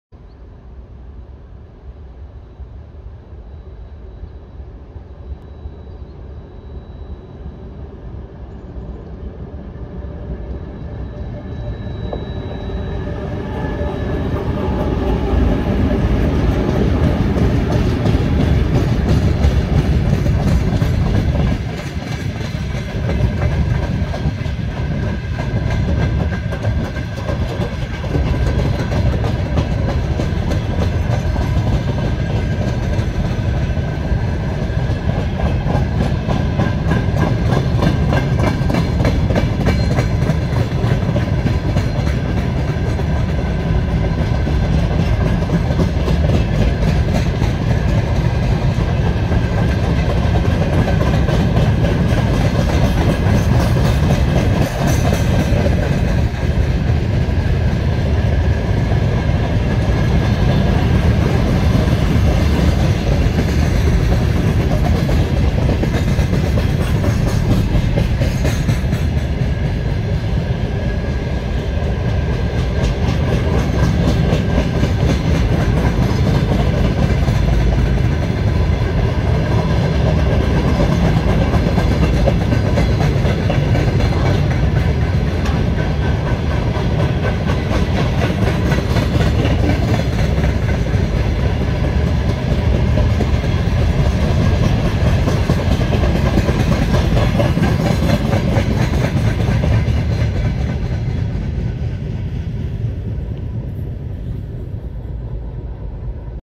2M62M- 1161 with a freight train
Lithuanian railways (LTG-Cargo) with a freight train 2M62M- 1161 between Jonava- Gaižiūnai station. Lithuania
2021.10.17
Lietuvos geležinkelių (LTG-Cargo) prekinis traukinys 2M62M- 1161 tarpstotyje Jonavos- Gaižiūnų stotis. Lietuva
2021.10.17